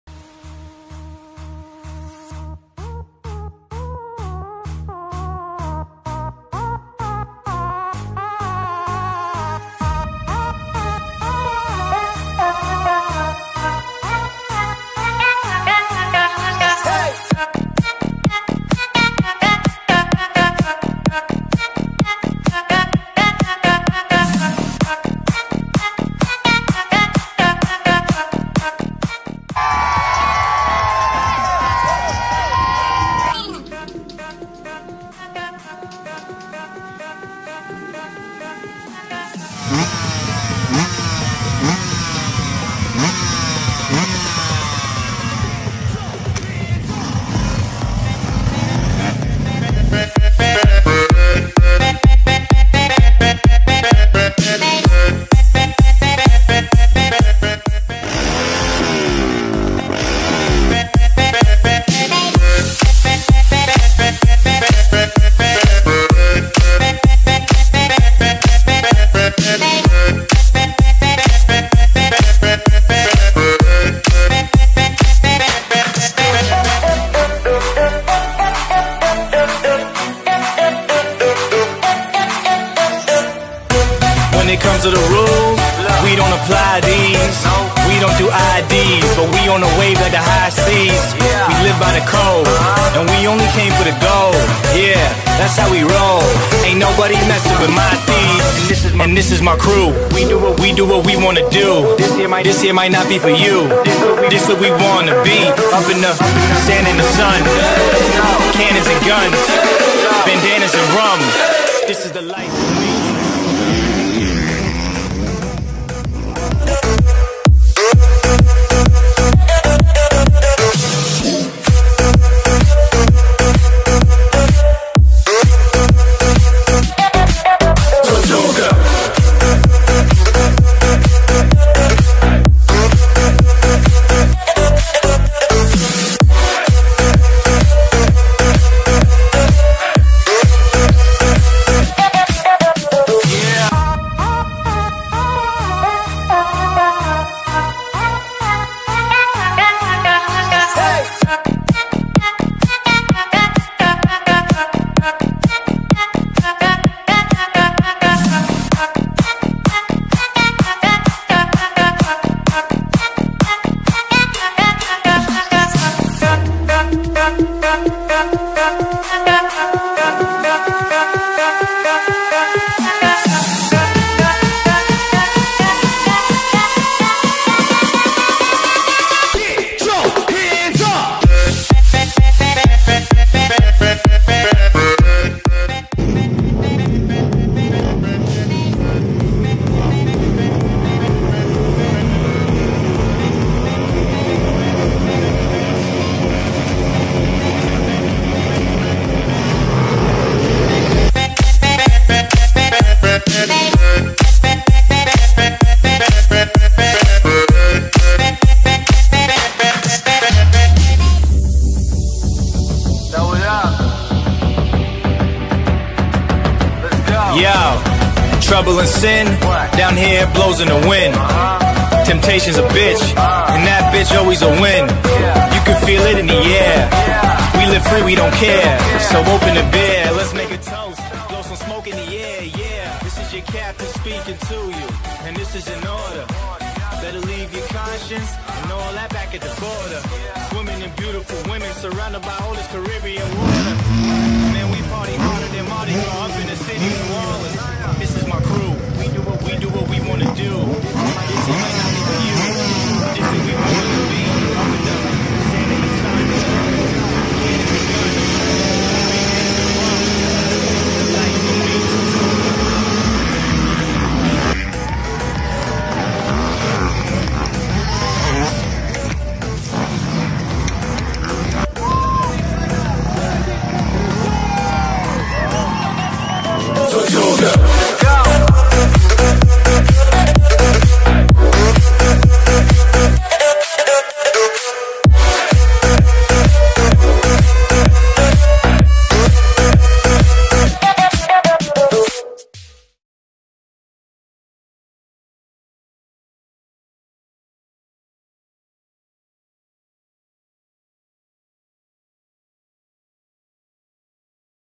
Ruff Riders Motocross 2019
Ruff Riders organized a Motocross Event at Thrissur, Kerala on 28 th April 2019

Video Courtesy : Hirosh P.G